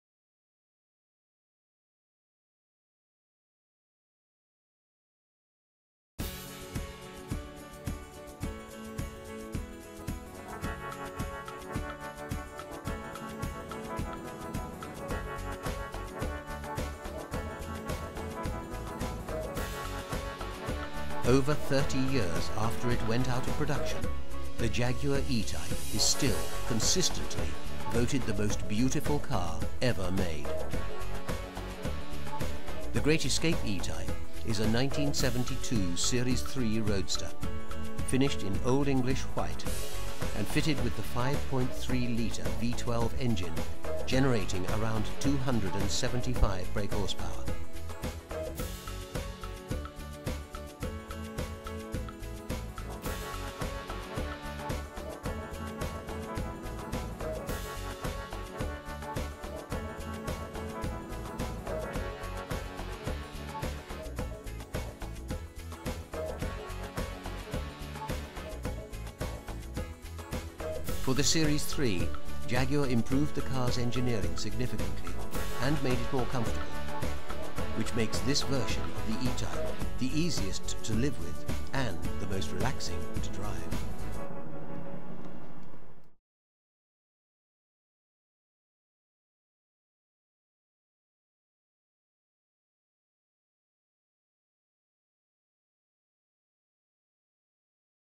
Jaguar E Type convertible in the Cotswolds
The Great Escape Classic Car Hire Jaguar E Type V12 convertible on the road in the Cotswolds. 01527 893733
Produced and shot by Warped Noise